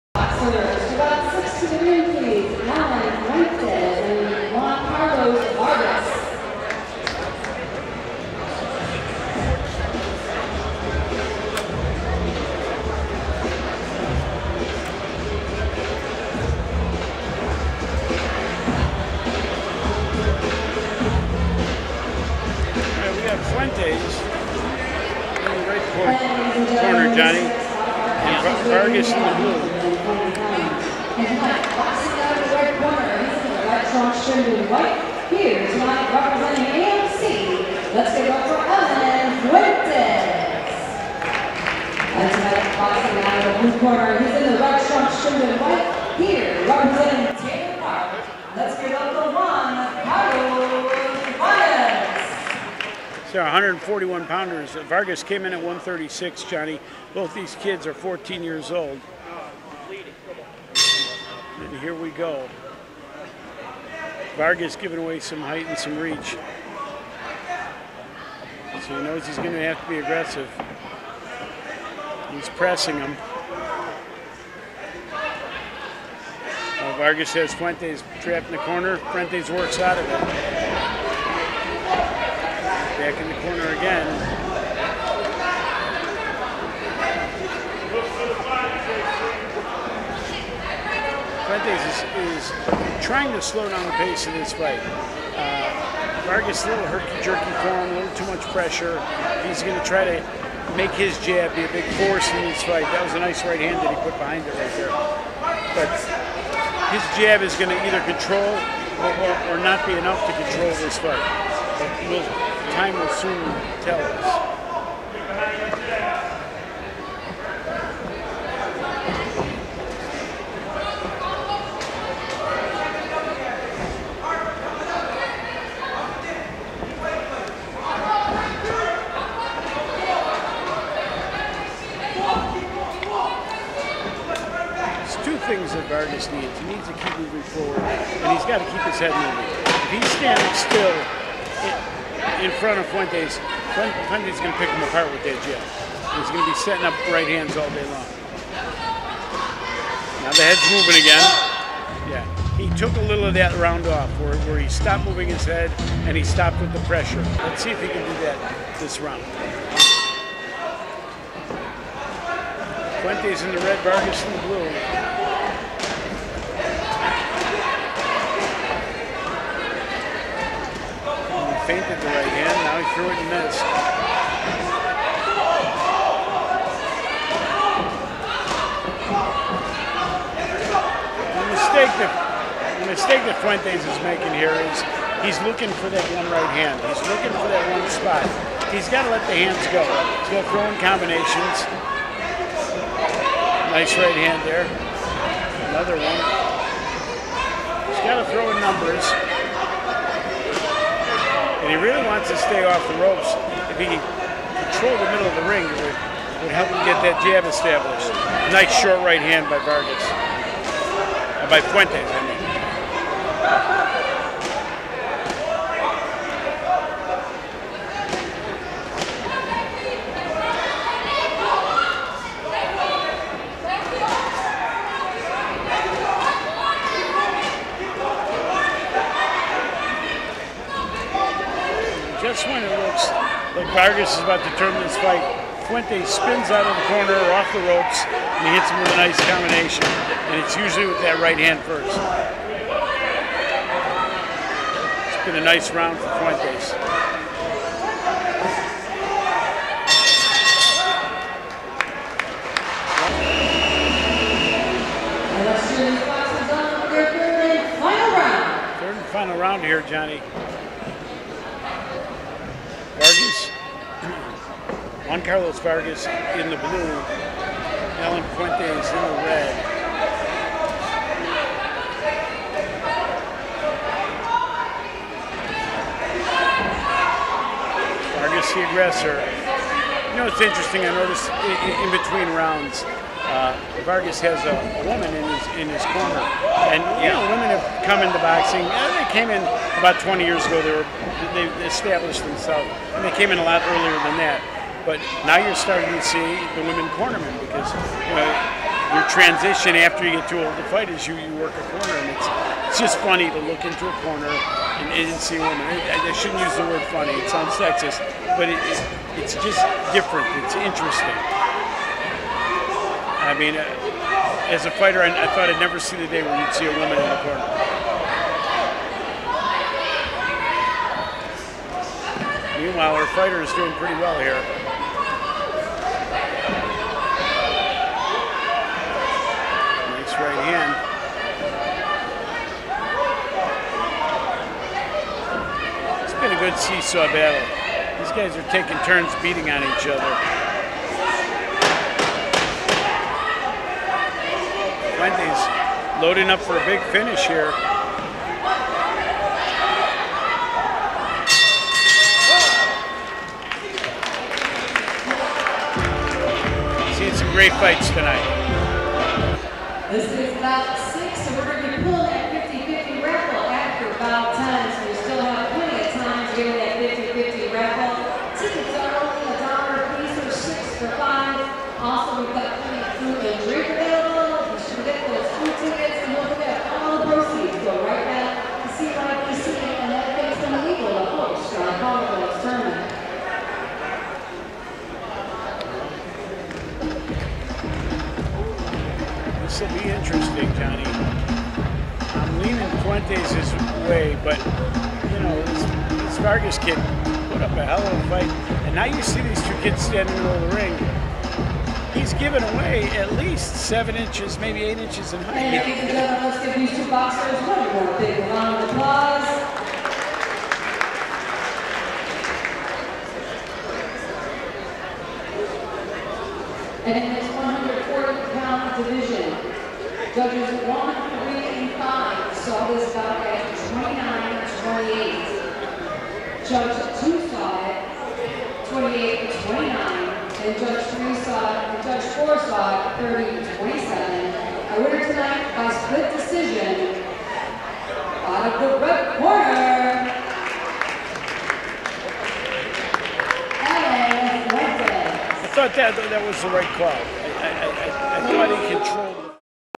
Alan Fuentes vs Juan C. Vargas 141LB. 6 of 73
Video of Chicago Fight Club Fights at Power Gloves